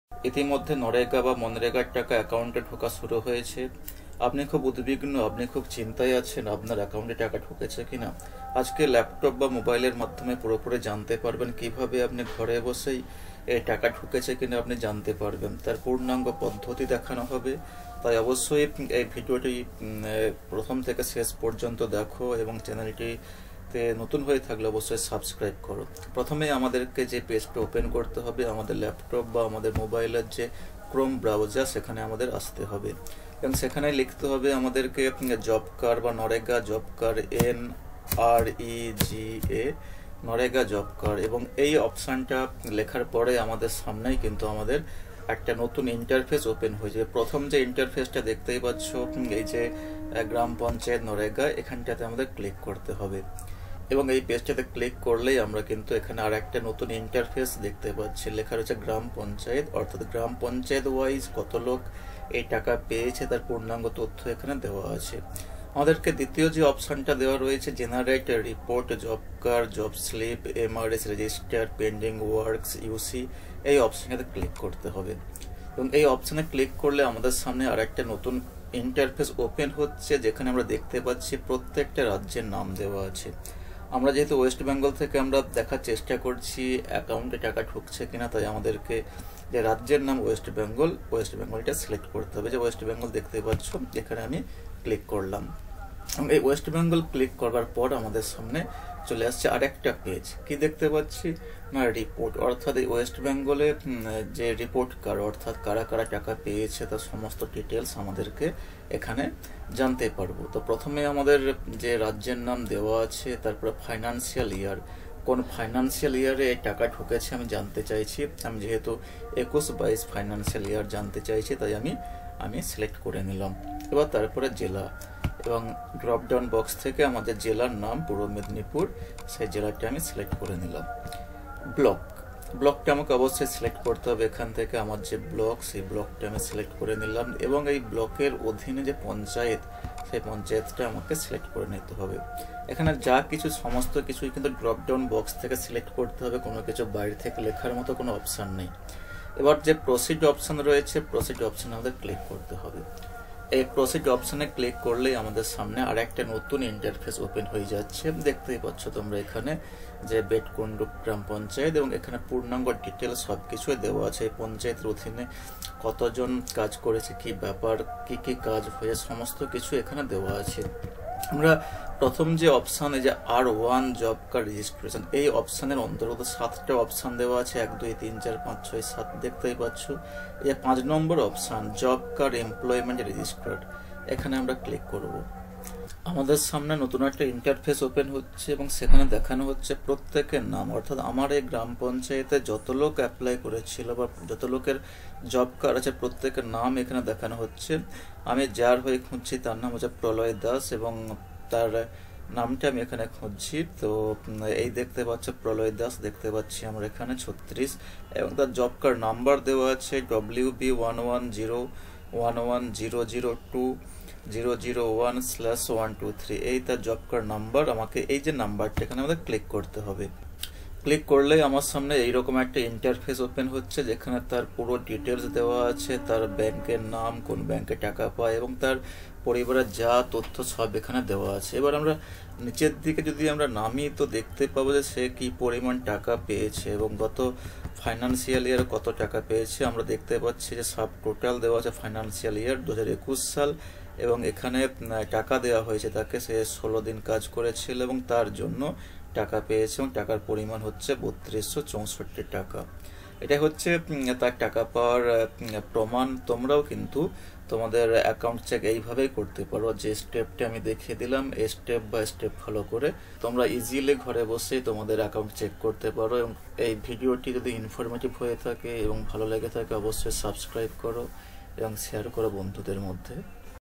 JOB CARD ACCOUNT CHECK:step-by-step লাইভ দেখানো হল কিভাবে একাউন্ট চেক করবেন
job card
mnrega cash credit